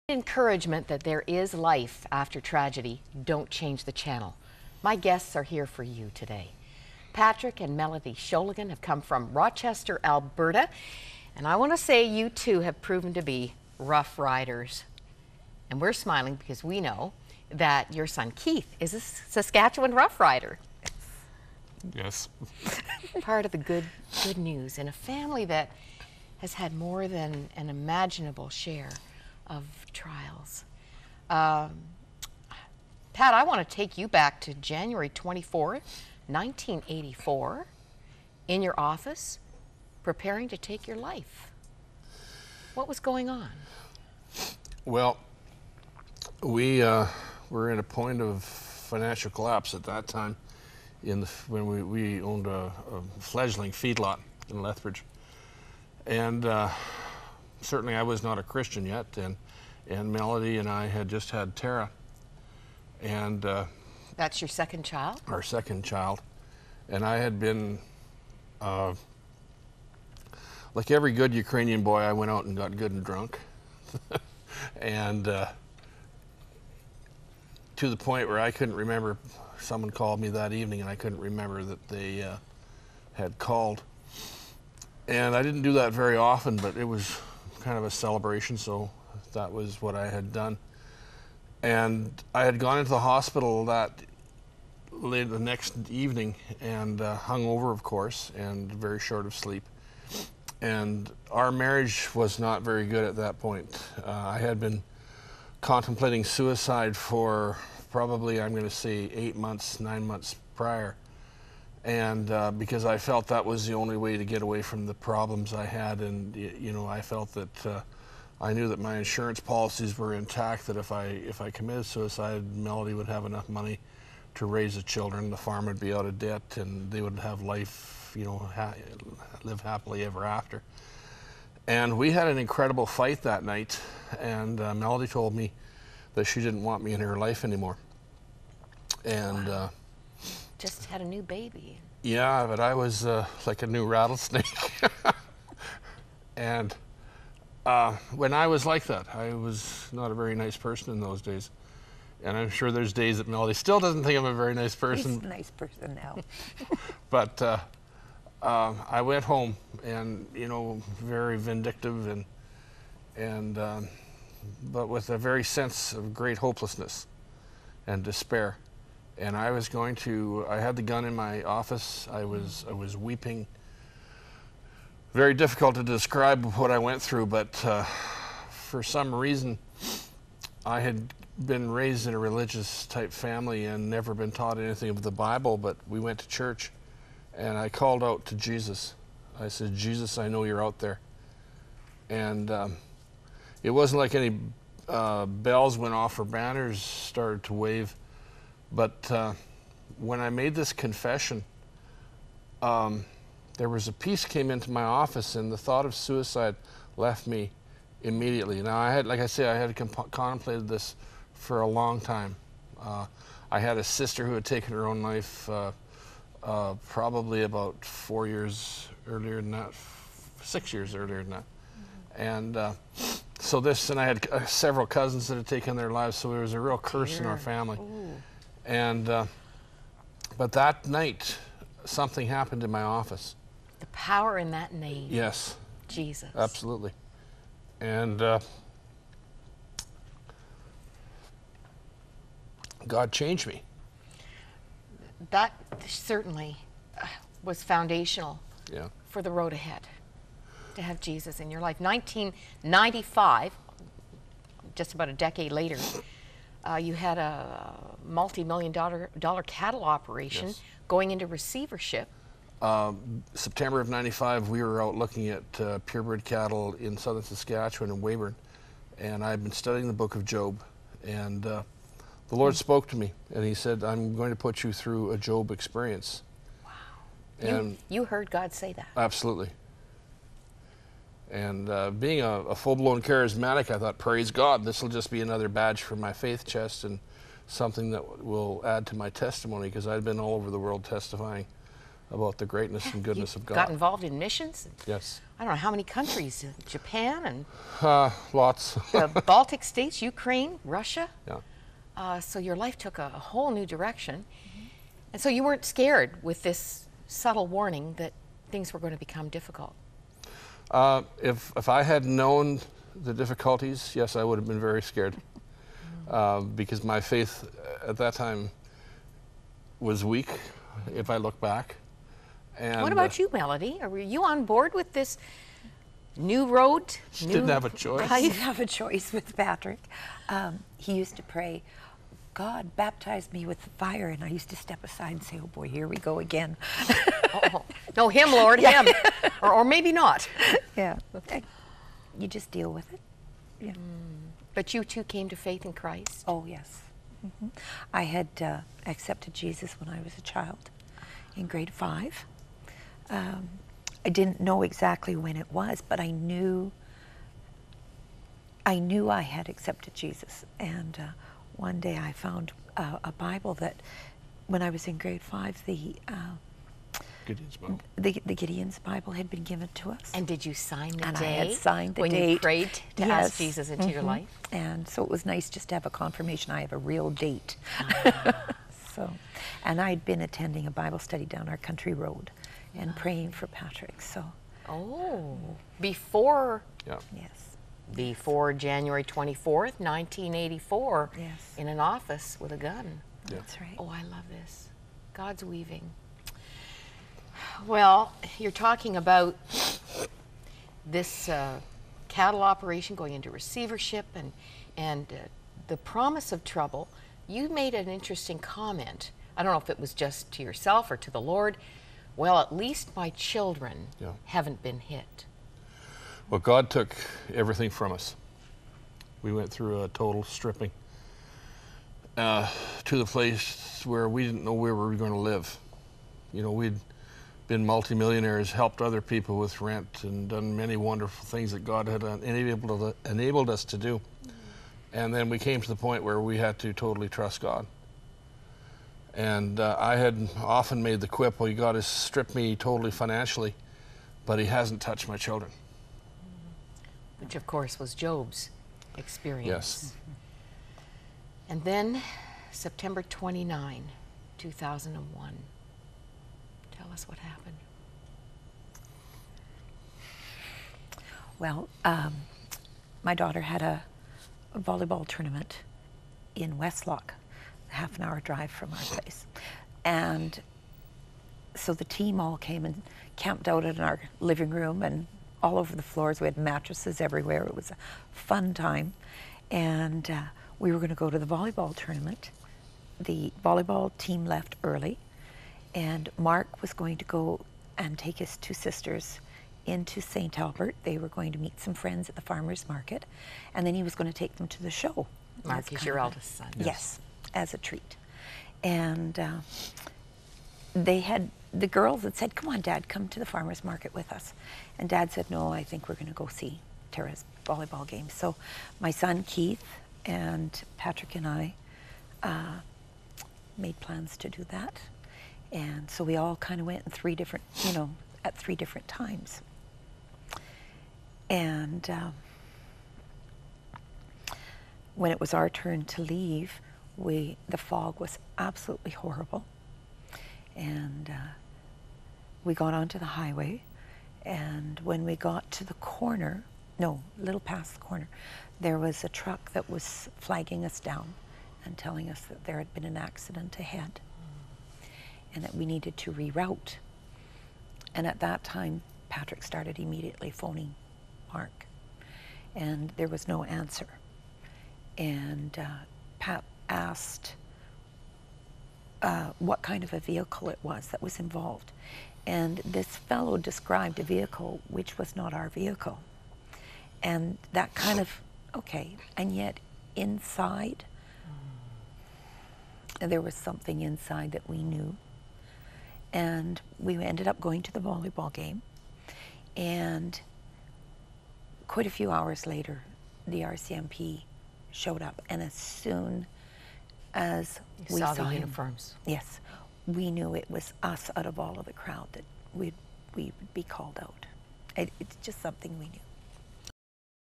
Overcoming The Loss Of A Child -- Patrick & Melody Shologan -- 1/2
Moira Brown speaks with Patrick & Melody Shologan about the tragic loss of their 2 young daughters in a 2001 car wreck in which their son was driving.
Patrick & Melody Shologan
Former Northern Alberta Cattle Ranchers